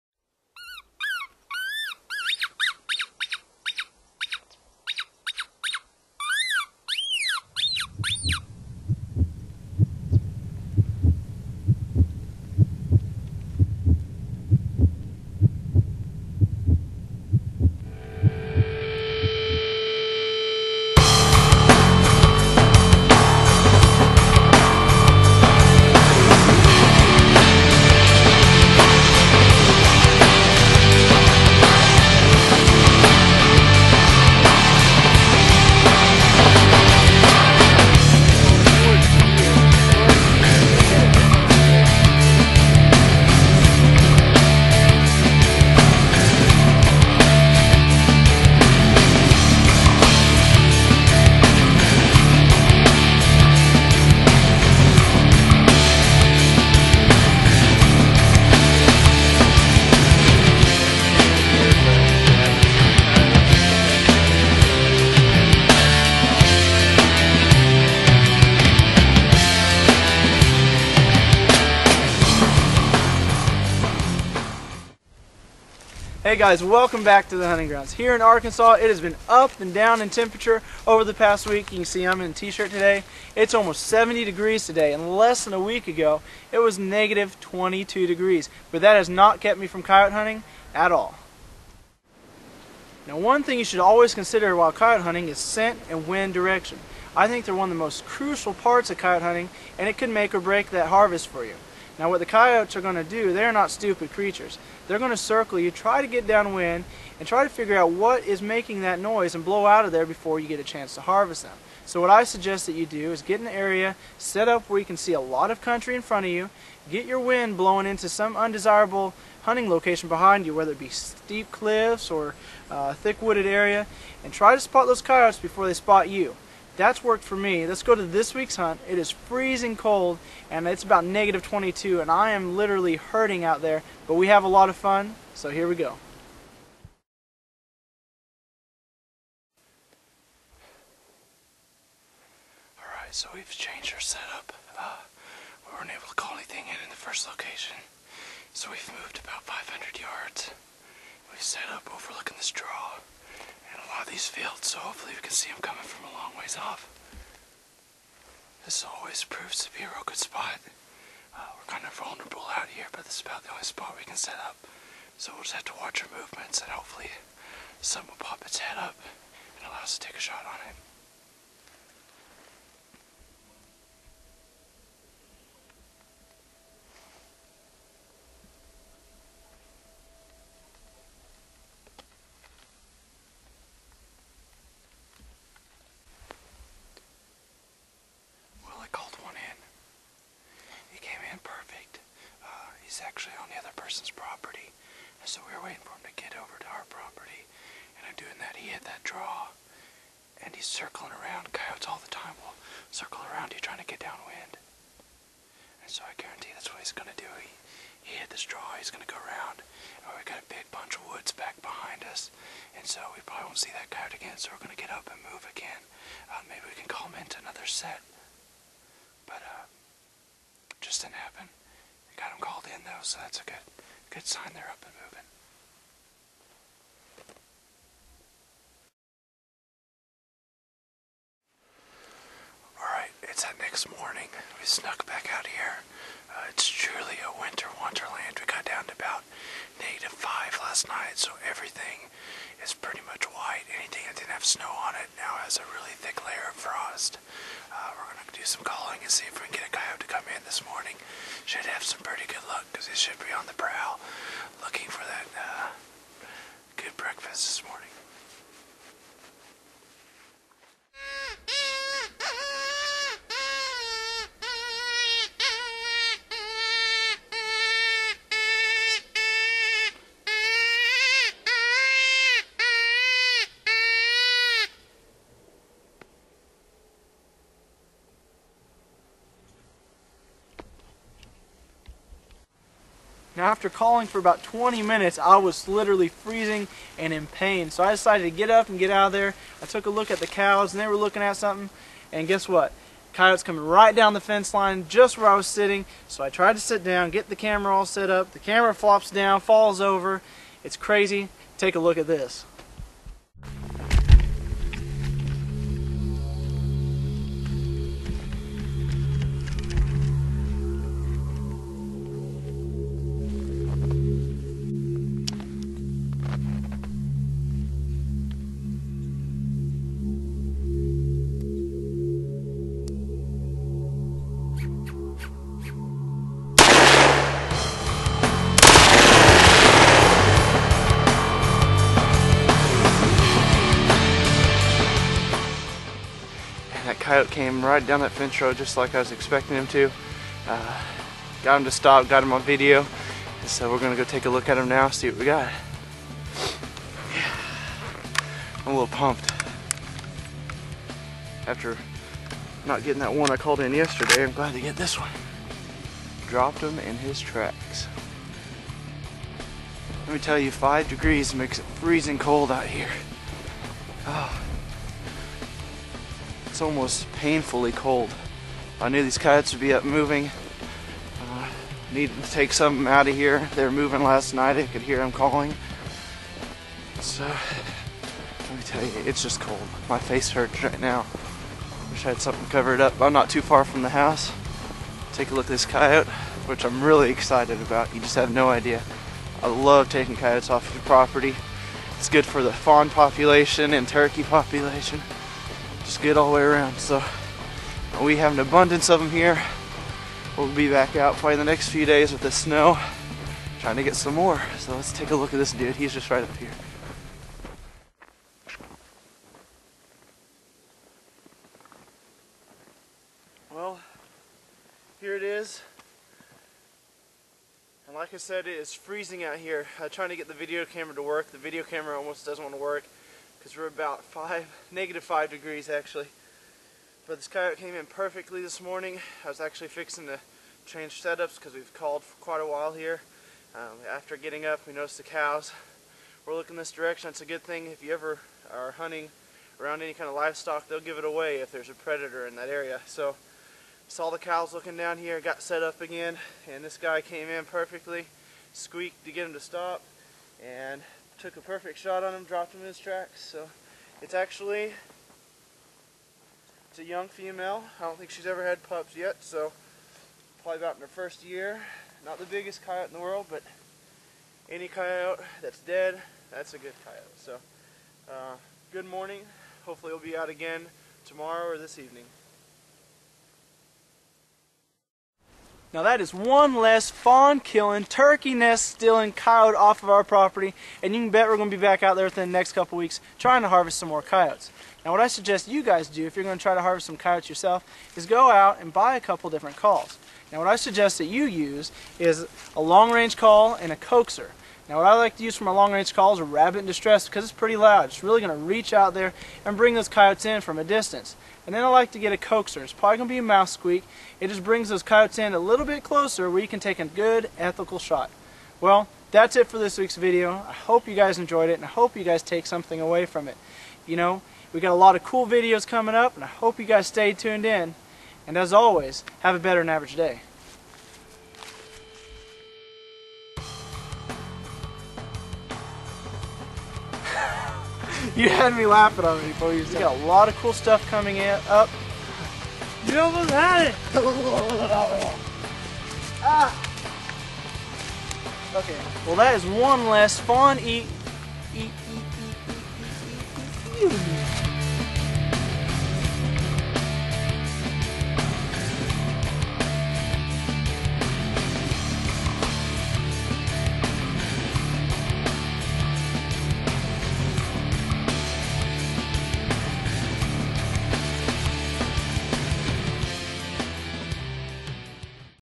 Coyote Predator Hunt : The Huntin Grounds
Calling in Coyotes with the mouth call.  I teach you the techniques I use to call them in.  It's by far my best video and we have some cool encounters.

WE'VE NOW GONE TO AN HD ONLINE SHOW!!!  We release a NEW Episode every two weeks on hunting/management.  Follow us Below: